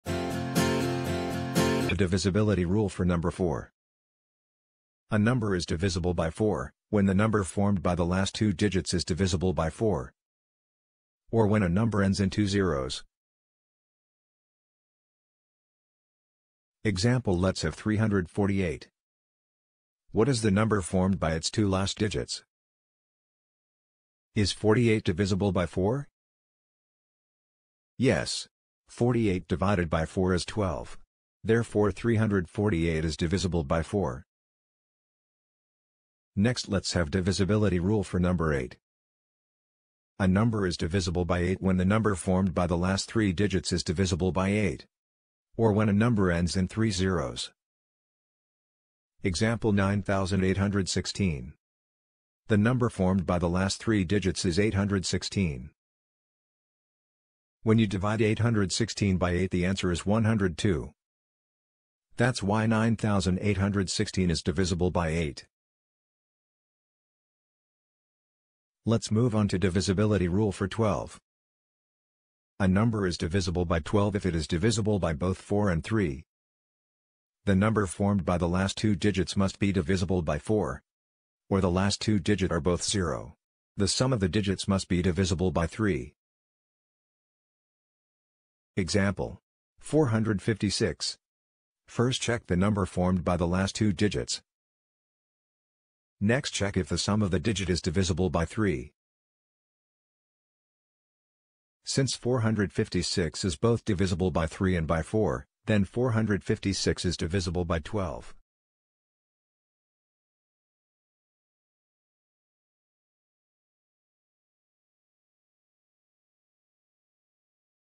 Divisibility Rules for 4, 8 and 12
A detailed explanation of the Divisibility Rules for 4, 8, and 12. A tool for parents and teachers to assist their kids.